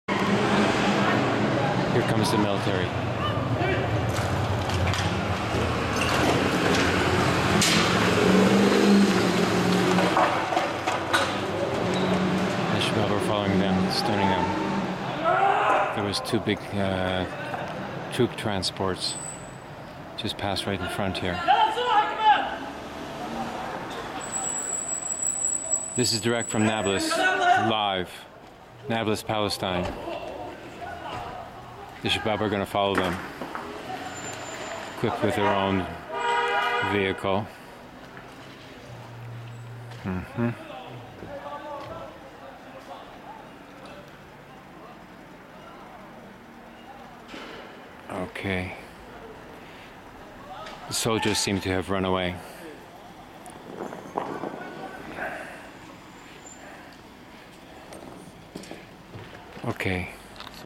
Military in Nablus night 2/2 الجيش في نابلس ليلا
Zionist military incursion into Nablus, city during night on downtown street Shara Hattin. Nightly incursions are regular occurrence  for the purpose of arresting Palestinian militants, activists, demonstrators, organizers academics, journalists even under administrative detention without charge. No law applies to the West Bank including the so-called Sector A like the large municipalities. Only military martial law is applied. No Israel law, no international law, nothing but military decree with recourse before a military tribunal and not a court of law. The military tribunal is subject to the judgment of a military officer and not a judge.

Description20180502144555